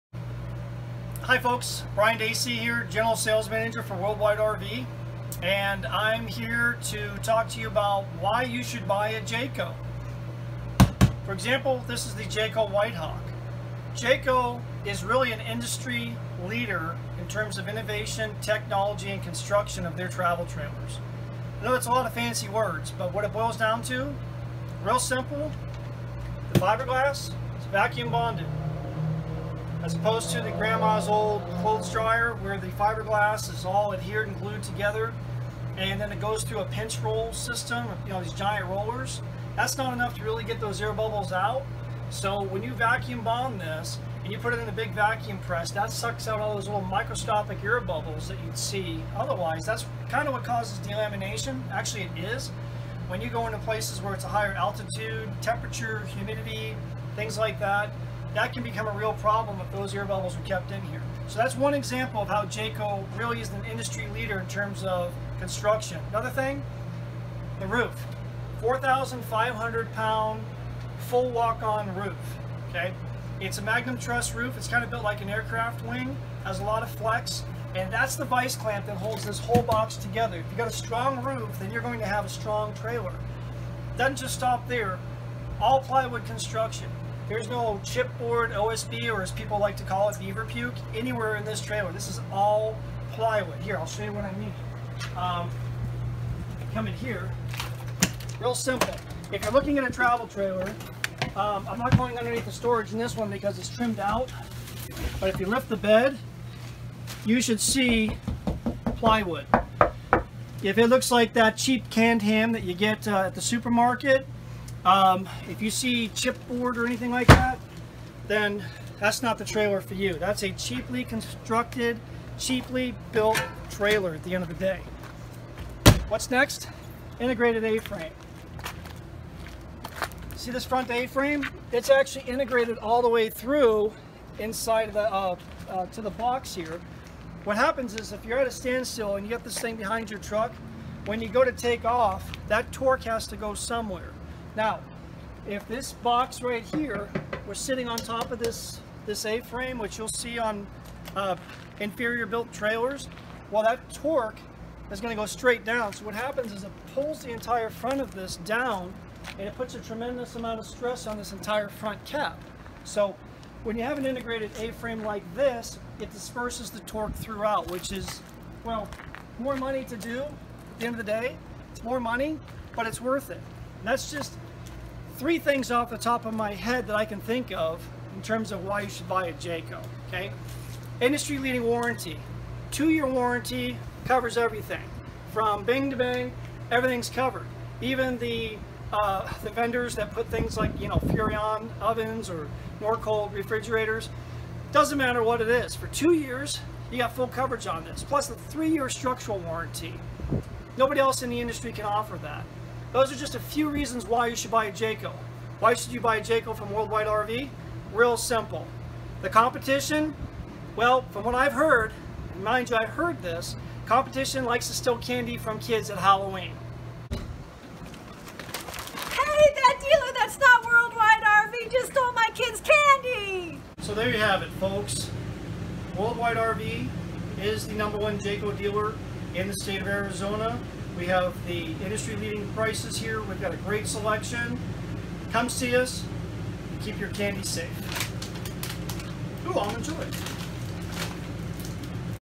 Why buy Jayco at World Wide RV
Why buy Jayco? Why buy from World Wide RV? General Sales Manager Brian breaks it down for you.